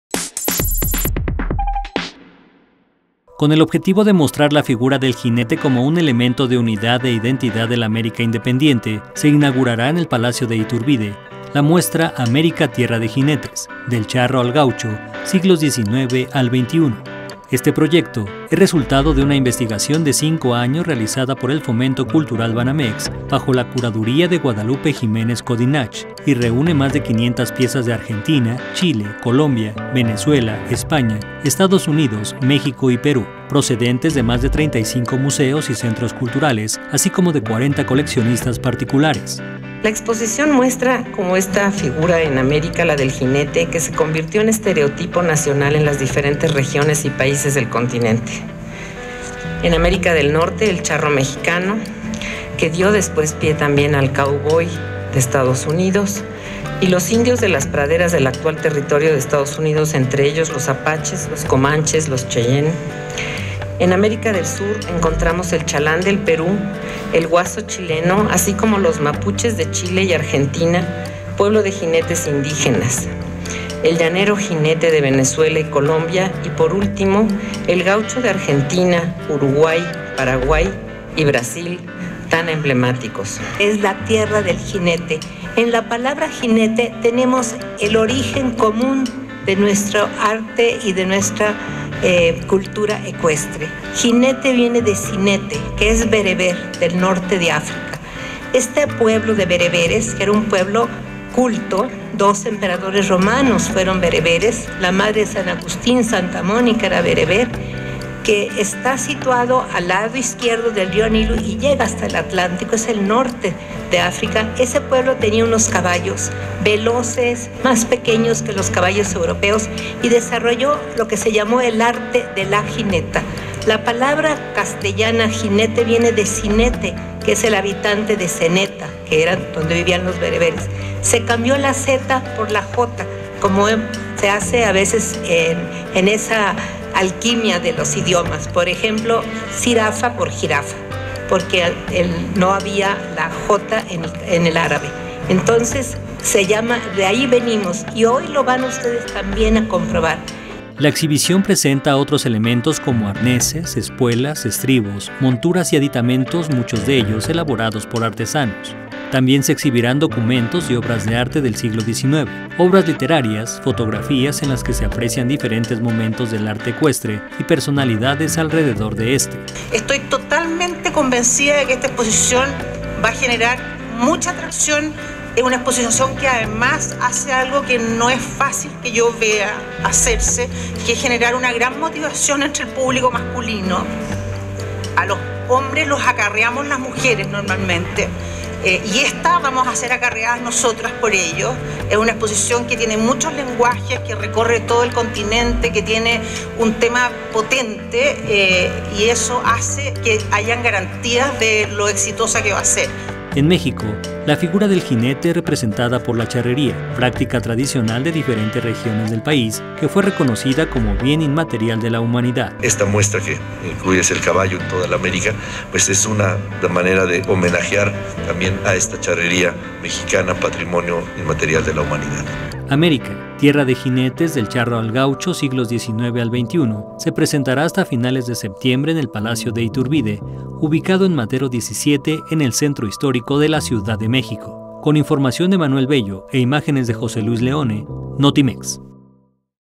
Cabalga en el Palacio de Iturbide la figura del jinete en América
La exposición muestra cómo en América la figura del jinete se convirtió en estereotipo en las diferentes regiones y países del continente.